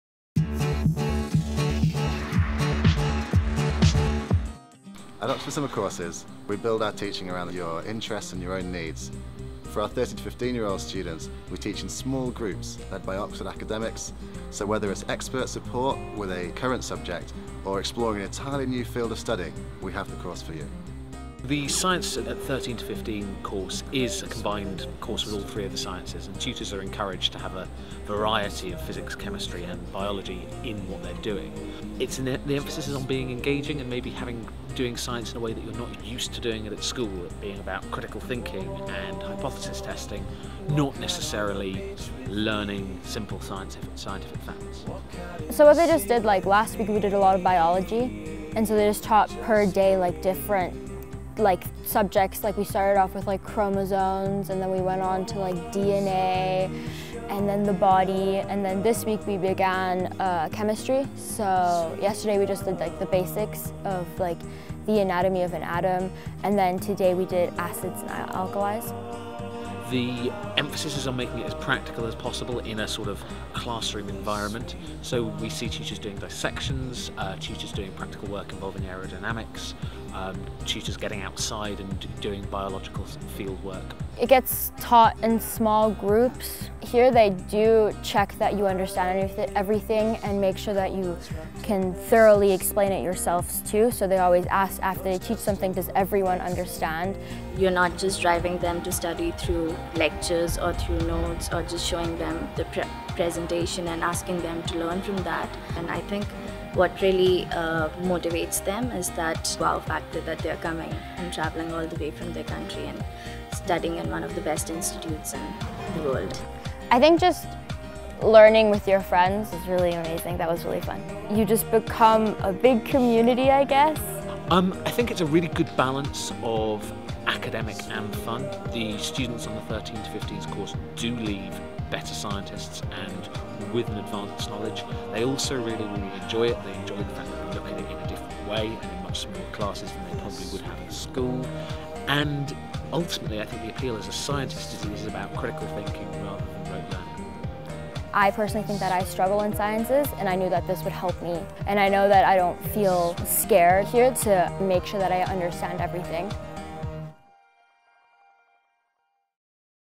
Science Course for 13-15 Year Olds
In a course specially designed for 13-15 year olds, you'll expand your knowledge and understanding of science through critical thinking and hypothesis testing. Taught in small groups, classes will expand beyond straight-forward facts and focus more on the scientific process, training you to think more critically about science by discussing the topics with expert teachers and your peers from around the world.